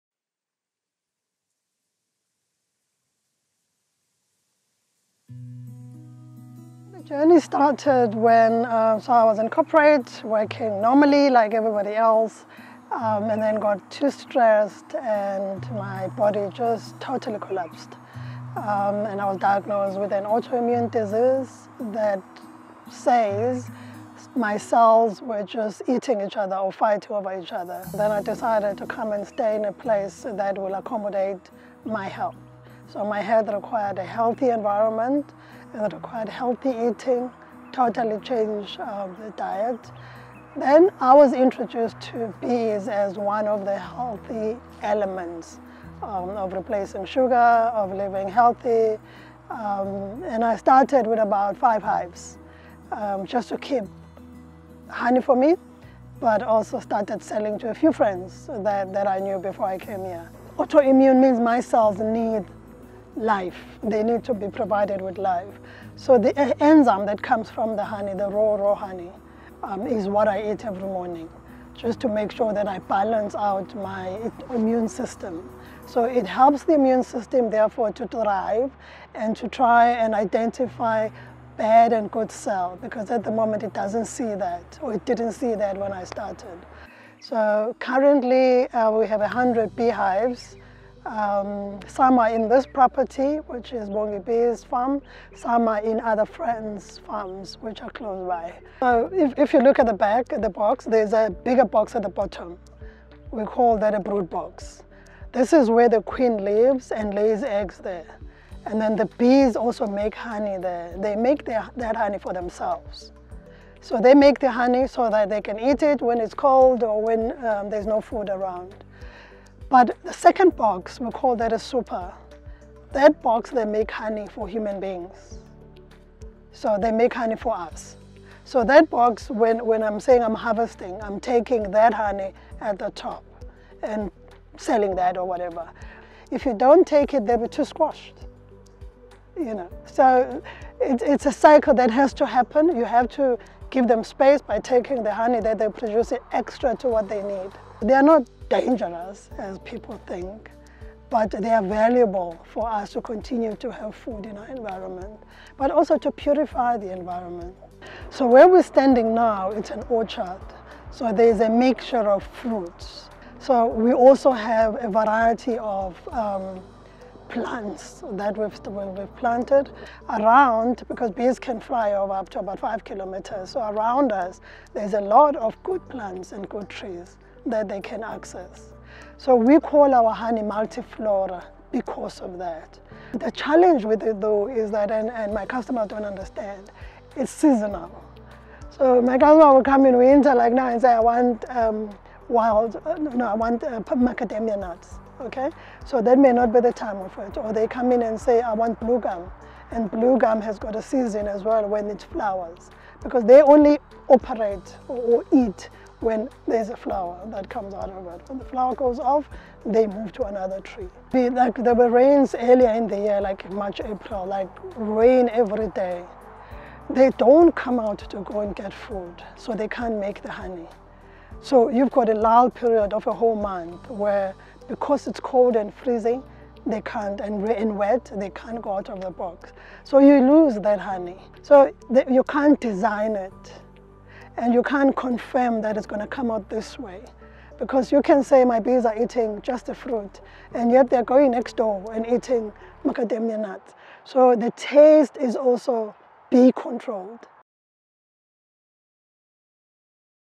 Beekeeper: Bees are helping me through a terminal auto-immune disease
In 2017 Lulama Lulu Letlape was diagnosed with an auto-immune disease, which caused her mind and body to crumble slowly. Her health forced her to change her eating habits and environment - to preserve her mind and body. Soon a mutual relationship began between her failing body and the endangered specie of bees.

By: Thahasello Mphatsoe